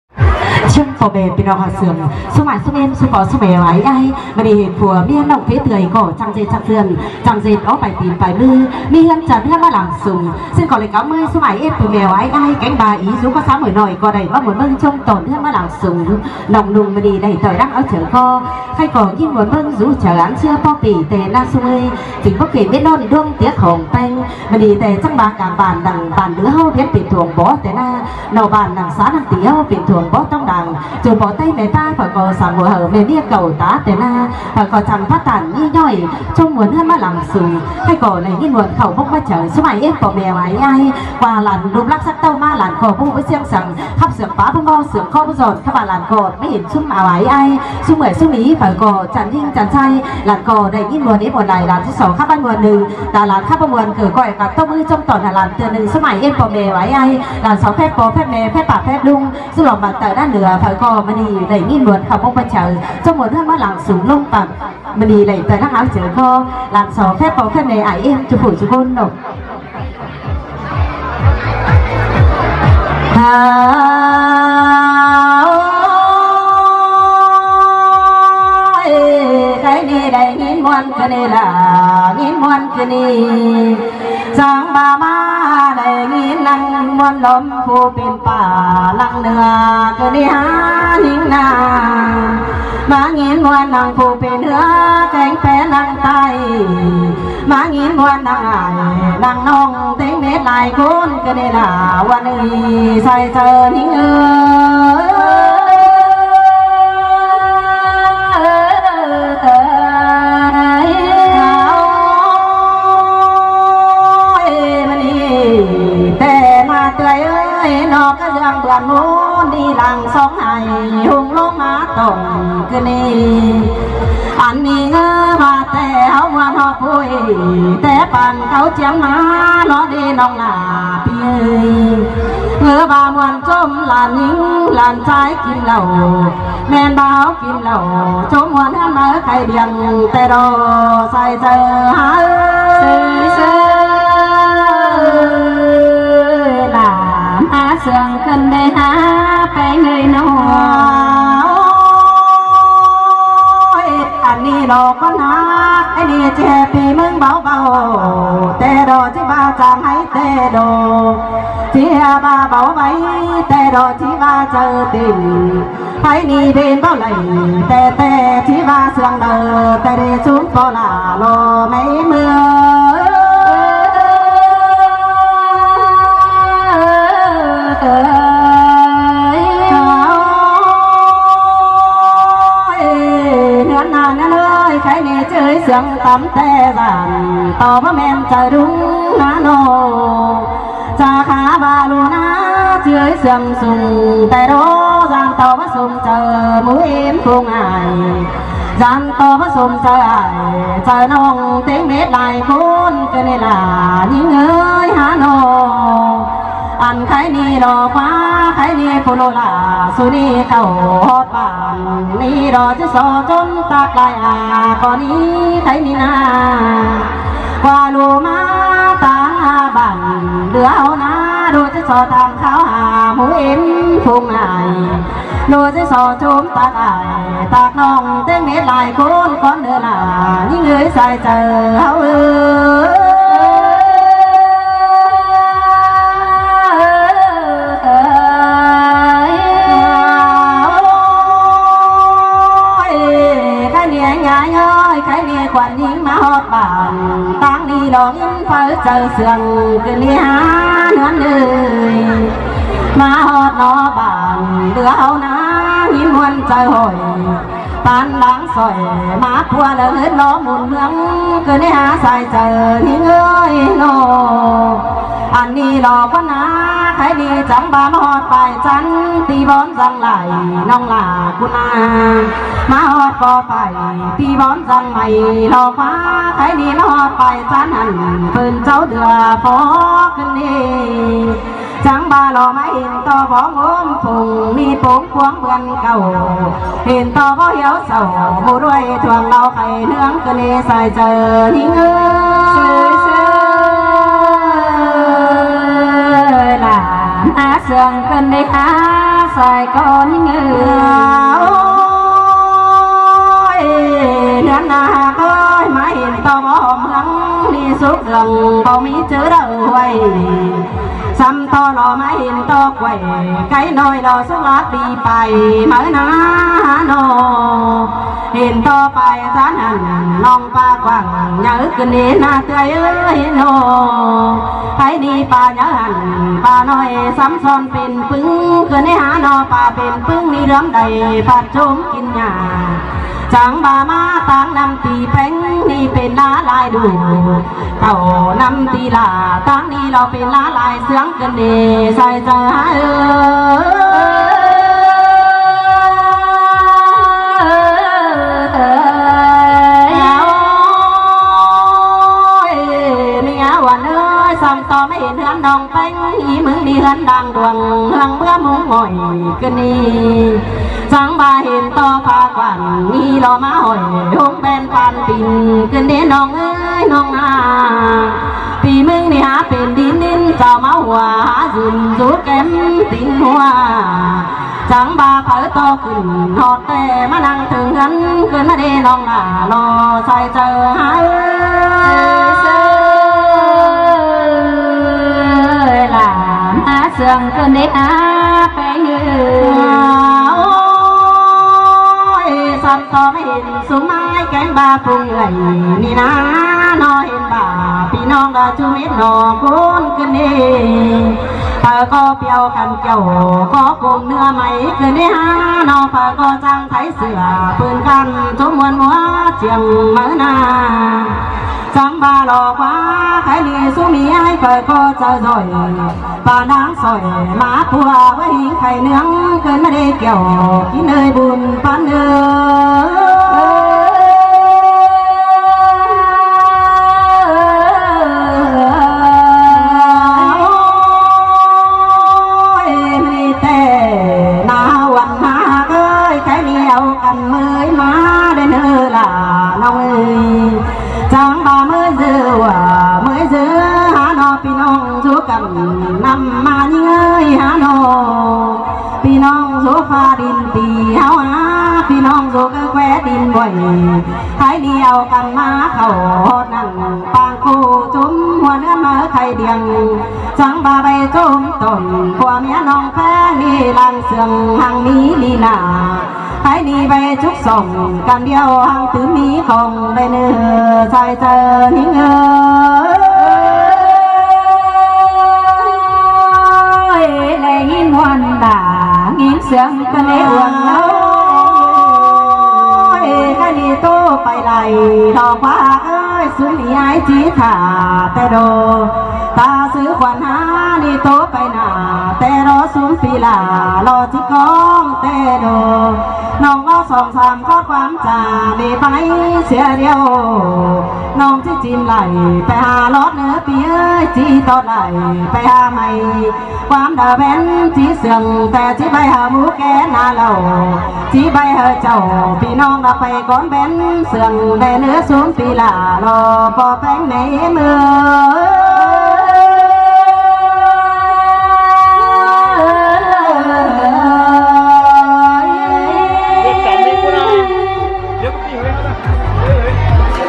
Lò Thị Thích Hát Mừng Nhà Mới Hay nhất | Dân Tộc Sông Mã
Nghệ nhân lò Thị Thích Hát Mừng Nhà Mới MC Thế Tươi Bản Đứa Nậm Ty sông mã Sơn la